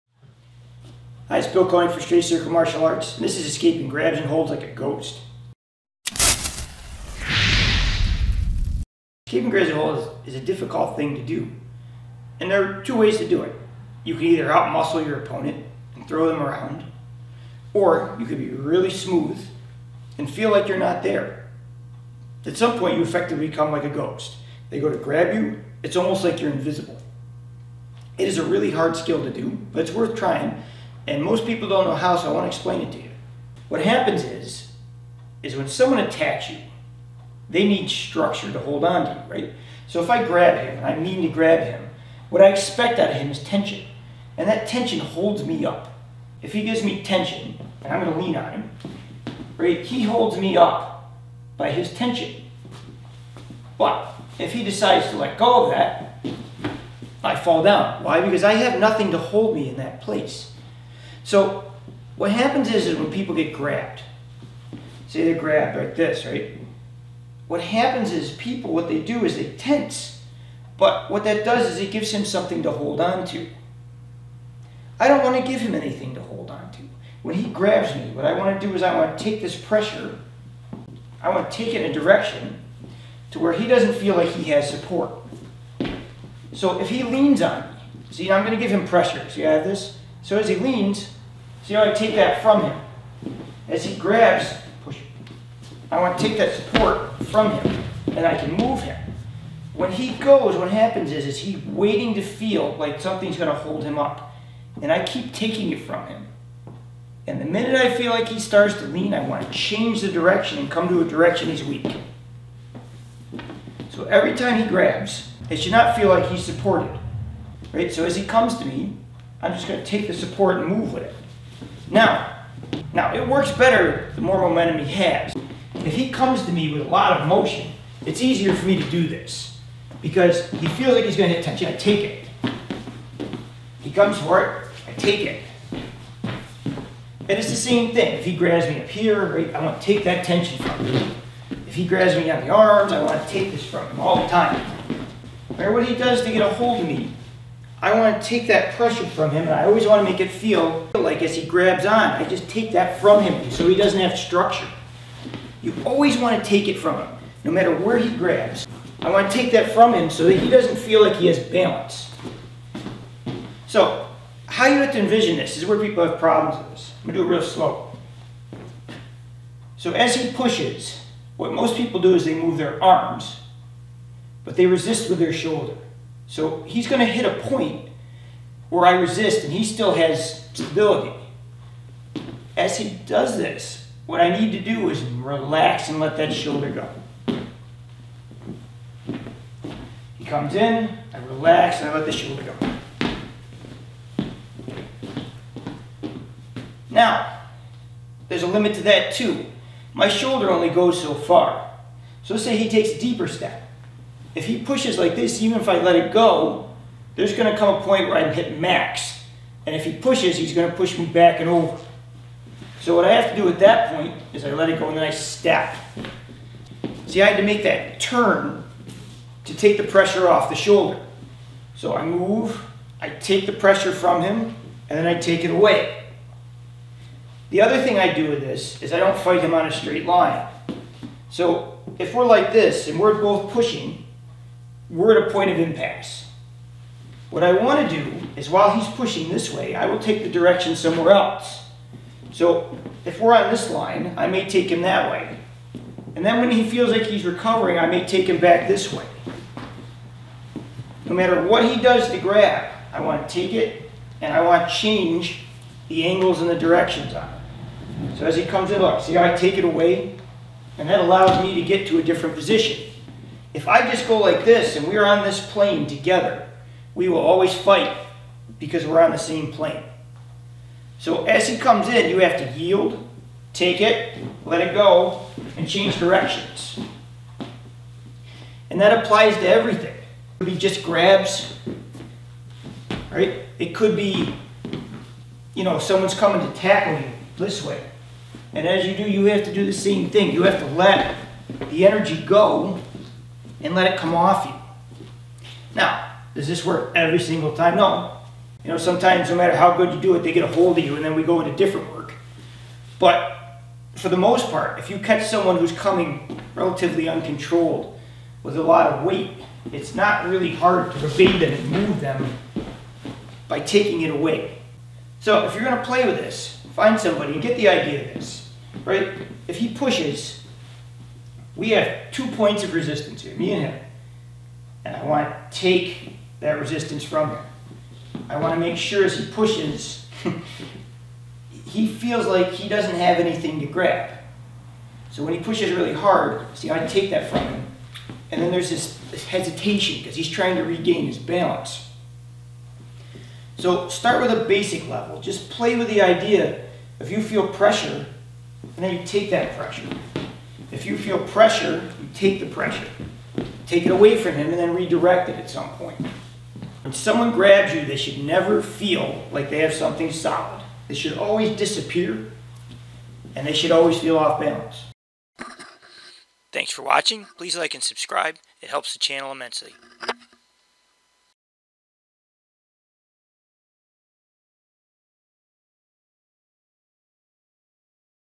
Escape like a ghost in a fight.
Learn how to make your attacker feel like your unhittable.